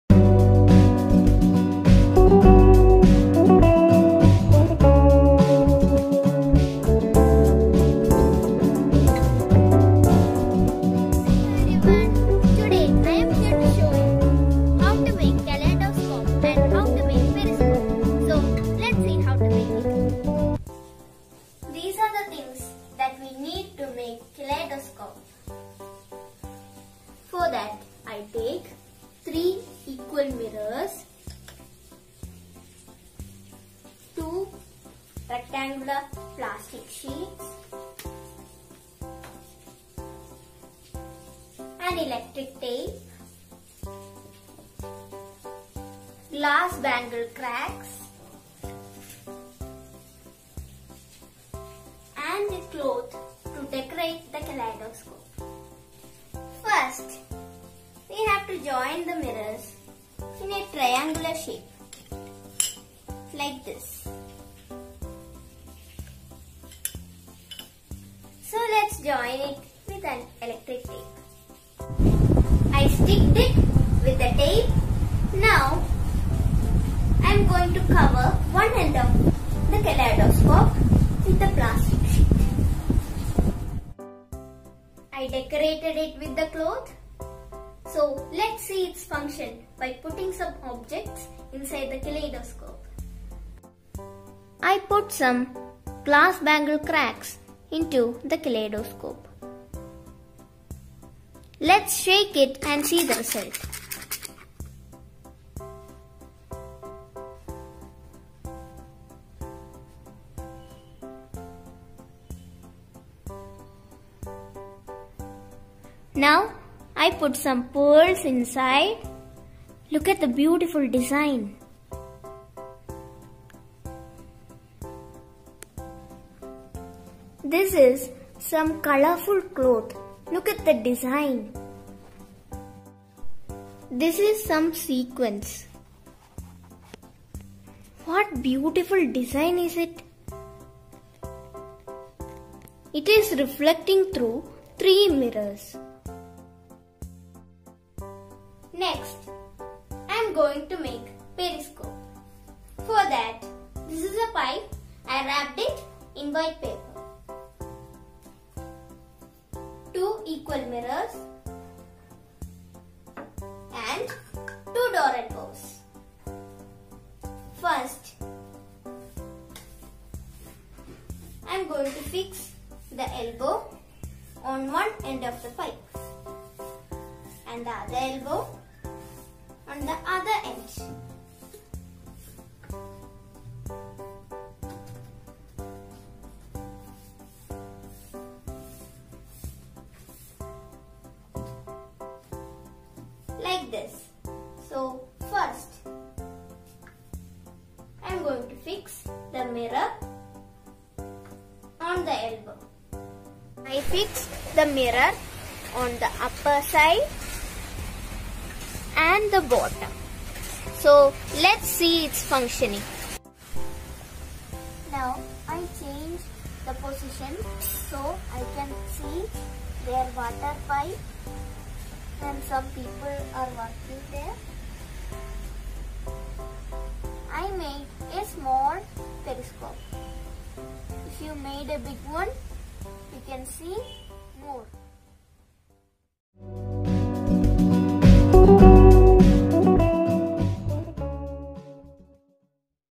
Learning aids based on the multiple reflection of light I Science experiment I Science Malayalam
Making of kaleidoscope and periscope